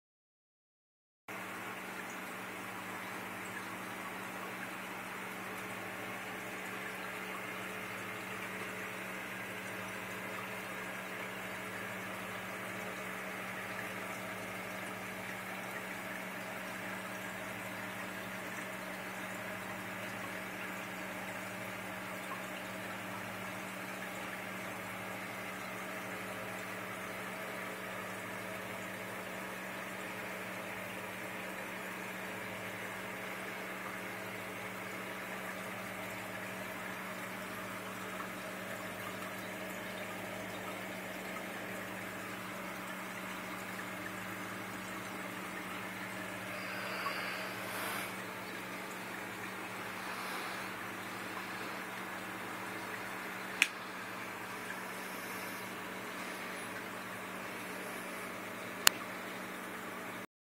50 Gallon Rainbowfish Aquarium
50 gallon fresh water aquarium with Rainbowfish and Gouramis.  (2) Dwarf Gouramis, (2) Turquoise Rainbowfish, (1) Boesemani Rainbowfish, & (1) Australian Rainbowfish